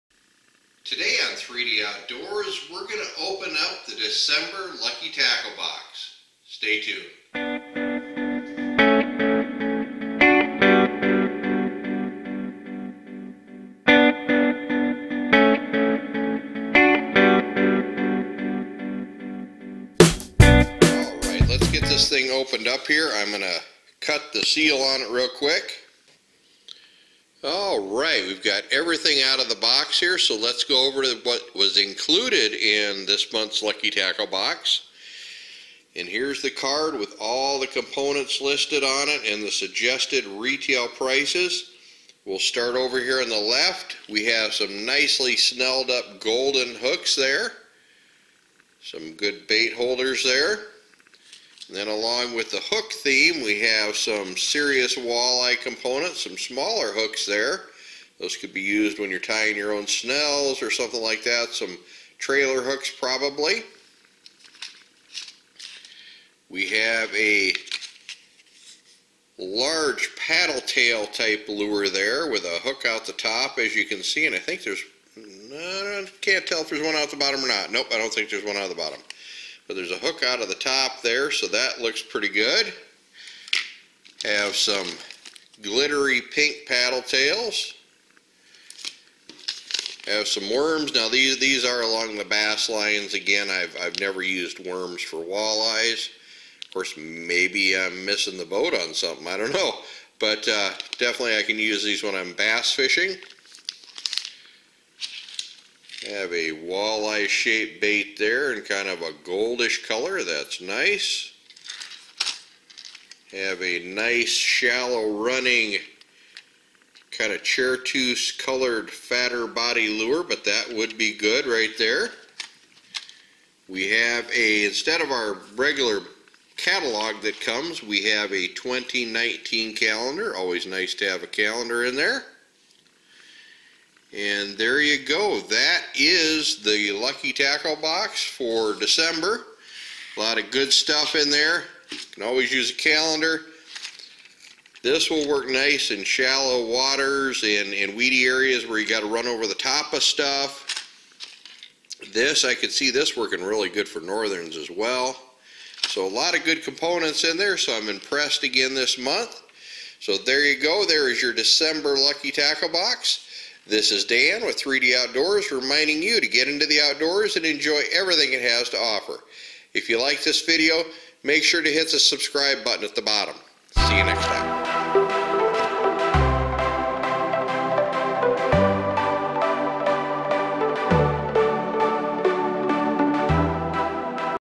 Lucky Tackle Box December 2018-Walleye Box
Today we are opening the December Lucky Tackle Box.. This looks like another good box with several items that I can use including a new calendar for 2019.